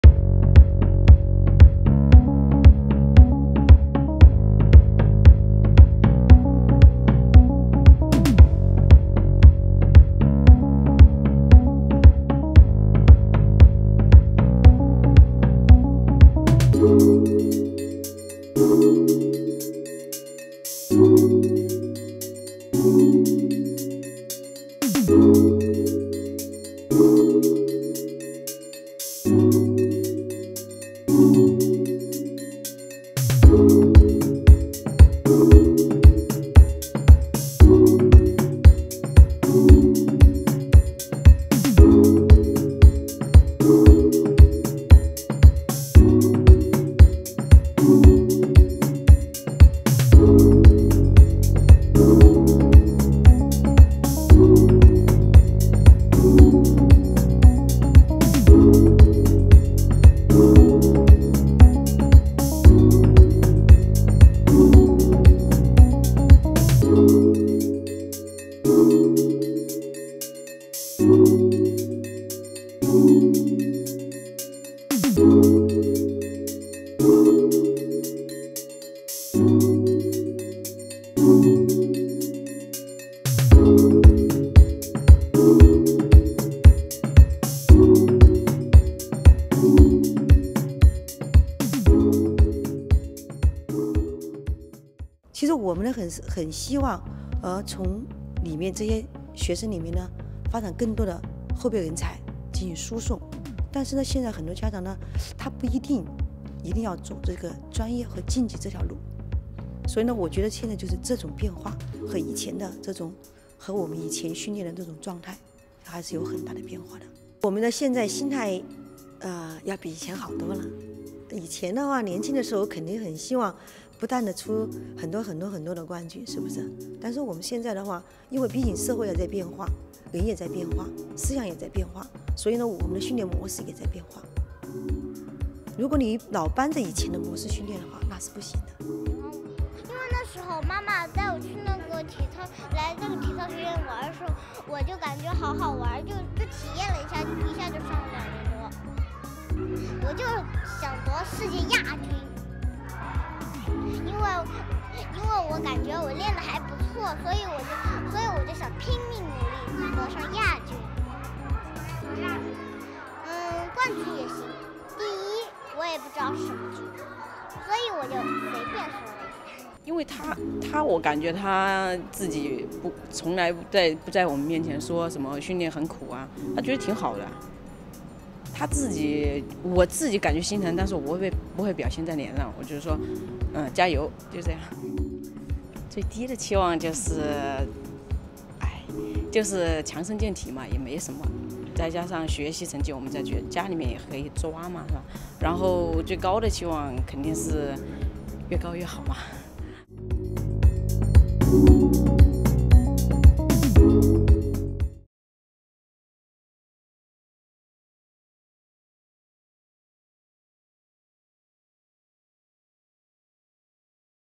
Inside China's gymnastics machine: the children training for Olympic glory
Under the twin emblems of the Chinese flag and the Olympic rings, two tiny boys dangle from the high bar in a cavernous gymnasium -- the cradle of China's elite gymnasts.They are among the latest recruits to China's notoriously demanding state sports system, which has attracted legions of critics but has also made it one of the most successful Olympic nations.